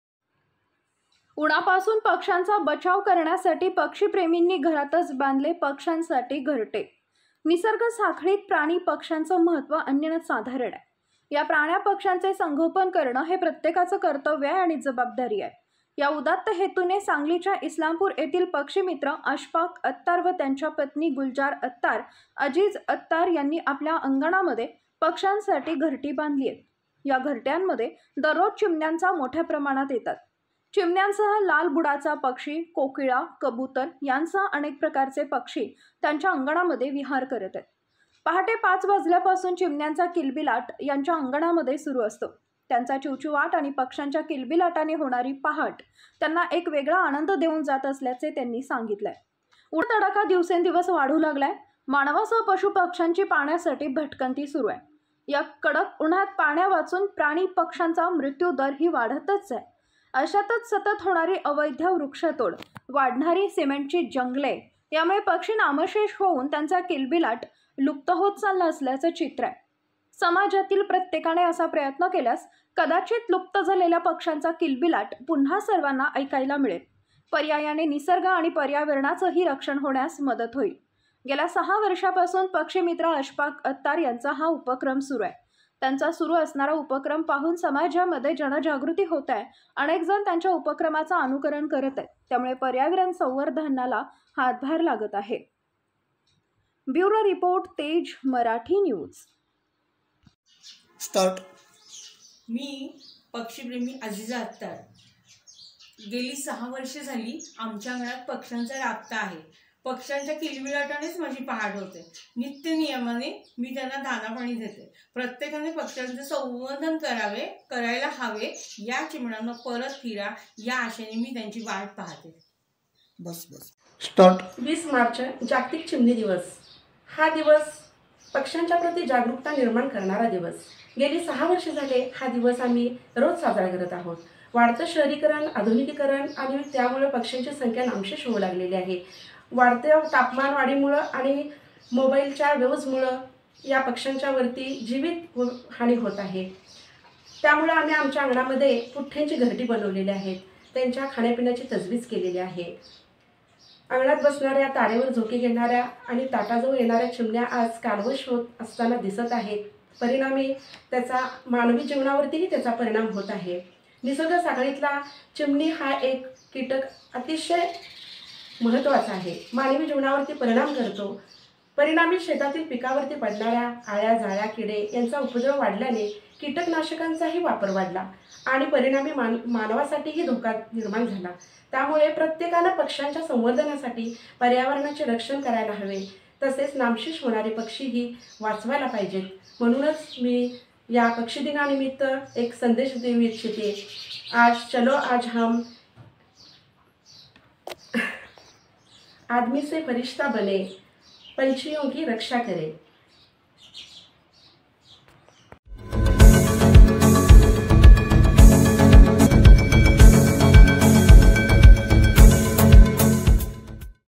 उन्हापासून पक्ष्यांचा बचाव करण्यासाठी पक्षीप्रेमींनी घरातच बांधले पक्ष्यांसाठी घरटे
SANGLI | उन्हापासून पक्ष्यांचा बचाव करण्यासाठी पक्षीप्रेमींनी घरातच बांधले पक्ष्यांसाठी घरटे